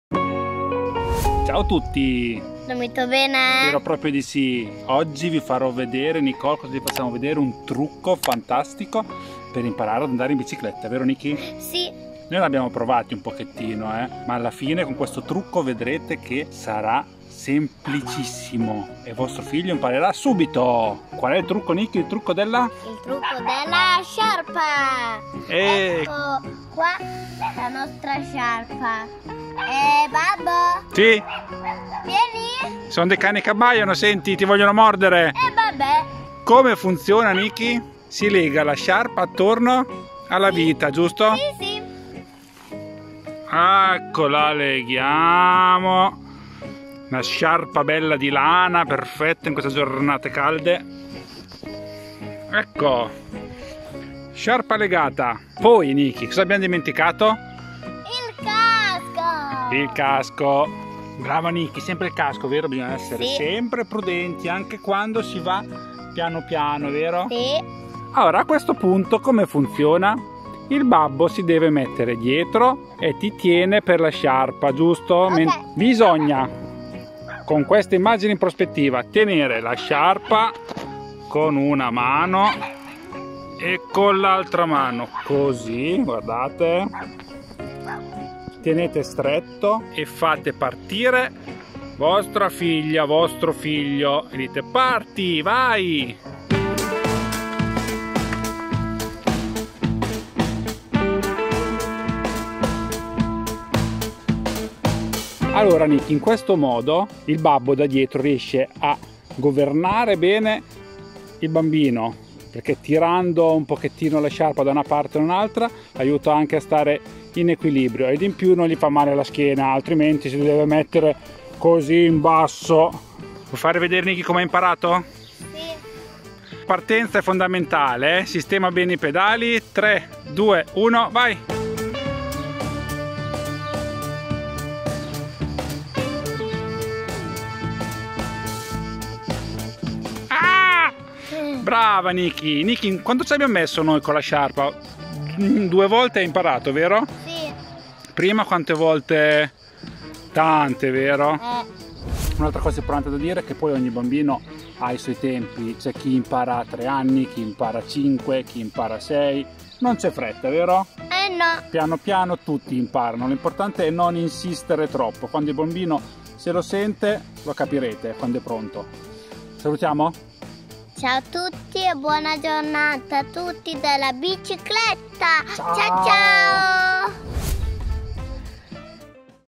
Imparare ad andare in bici con il metodo della sciarpa!
E' ora di togliere le rotelle!!!
Con il metodo della sciarpa imparare ad andare in bici sarà davvero semplice e divertente. Abbiamo provato tanti metodi, ma questo è risultato il più veloce, semplice, sicuro e pratico.